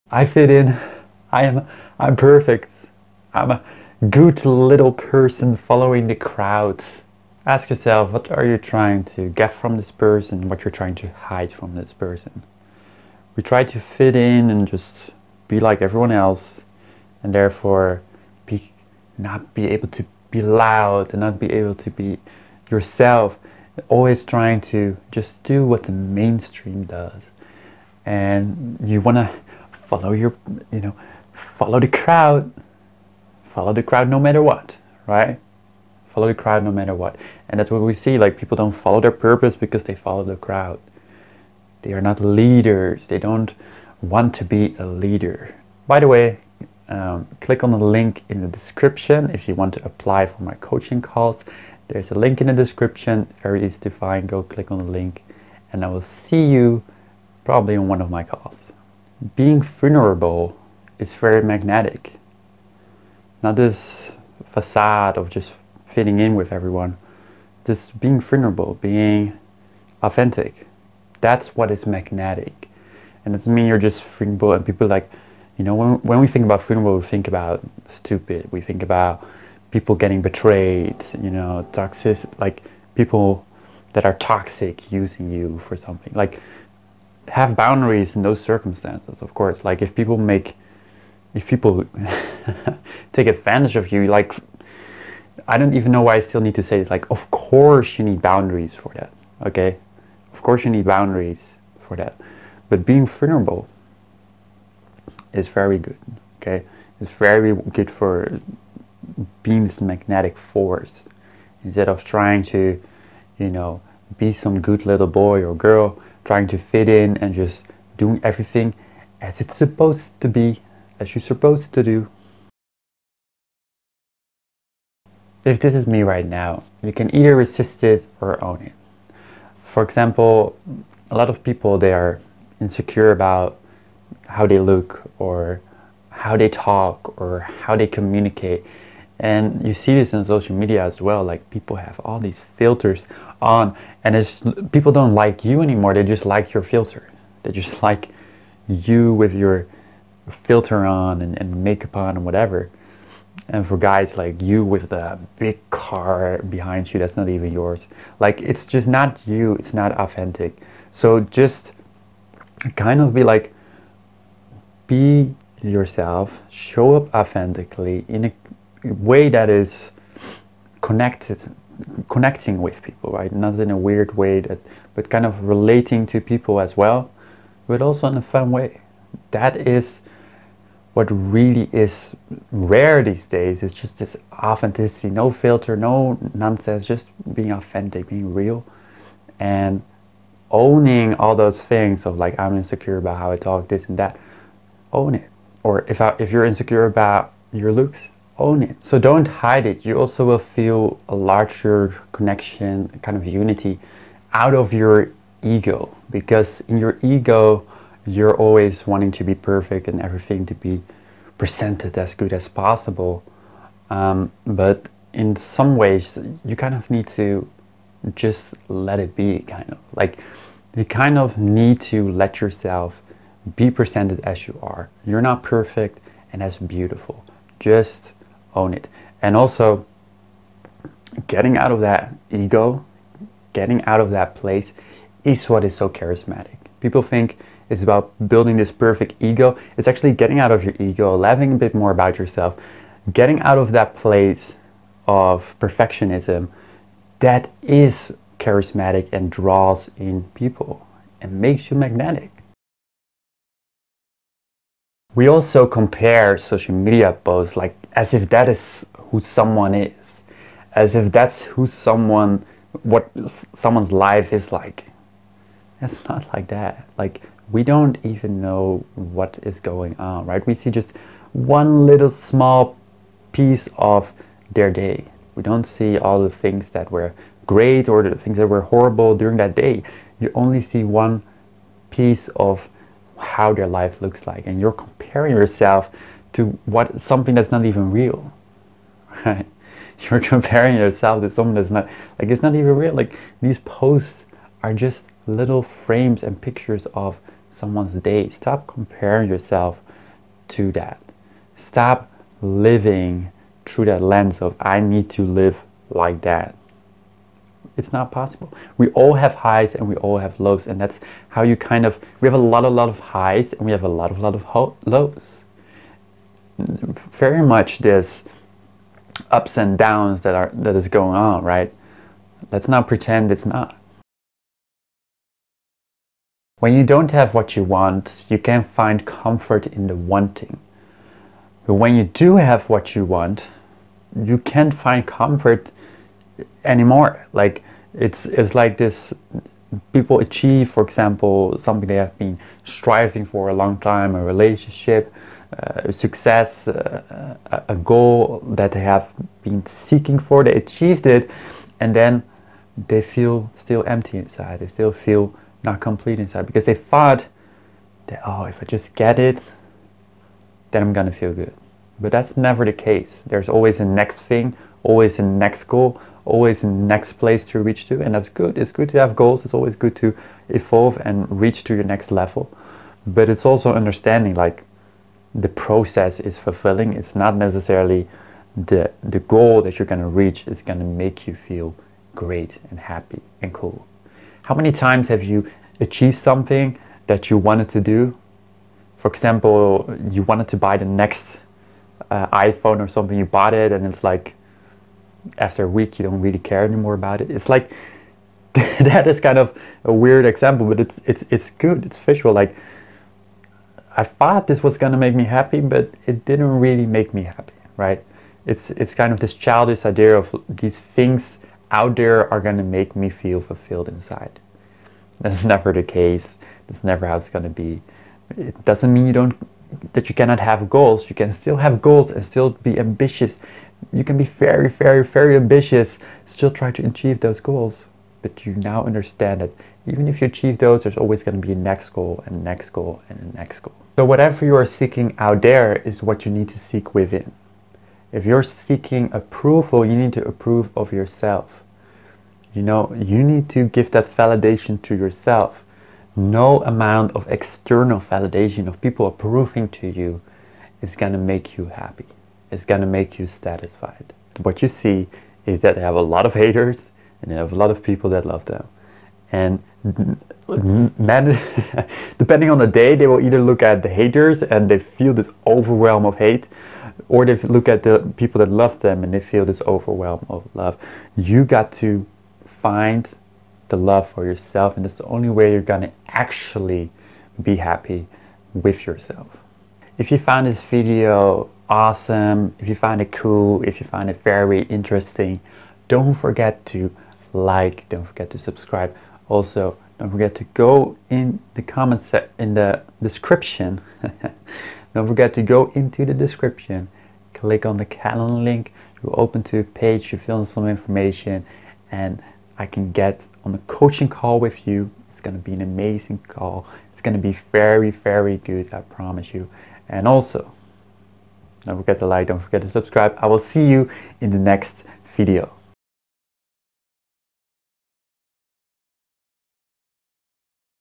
The Silent Hack That Makes People Magnetic
Have you ever wondered what sets magnetic individuals apart? In this eye-opening video, we uncover the silent hack that can transform your presence and attract positivity into your life. Comparison is the thief of joy. Learn powerful strategies to silence your inner critic and embrace your unique journey without constantly measuring yourself against others.

Social media can create unrealistic expectations. Dive into an honest discussion about the impact of filters and curated content on self-esteem and mental well-being. Seeking approval can be exhausting. Explore techniques to cultivate self-validation and confidence, freeing yourself from the need for external validation.

Authenticity is magnetic. Discover how to spot and gracefully distance yourself from fake individuals, allowing genuine connections to flourish in your life.